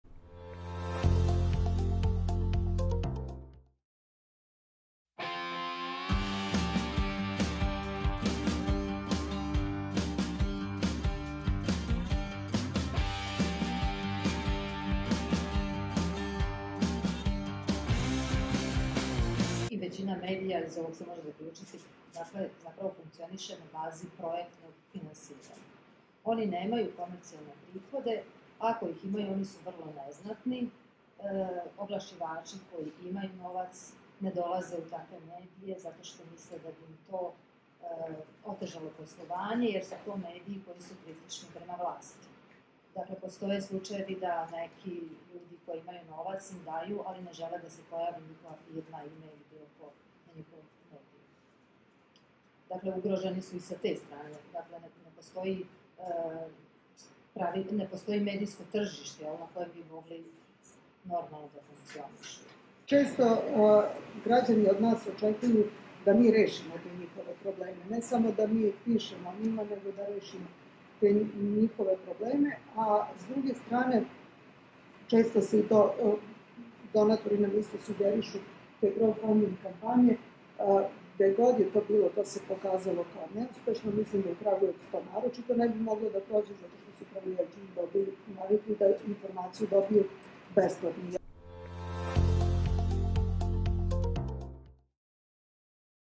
Tribina NDNV-a u Kragujevcu o funkcionisanju medija civilnog društva I deo
📌Javnu debatu u Kragujevcu je organizovao NDNV, u okviru projekta “Otporni mediji civilnog društva za obnovu poverenja građana”, uz podršku Delegacije Evropske unije u Srbiji.

Bilo je reči o značaju lokalnih medija za kvalitetno informisanje, kao i probleme u njihovom finansiranju.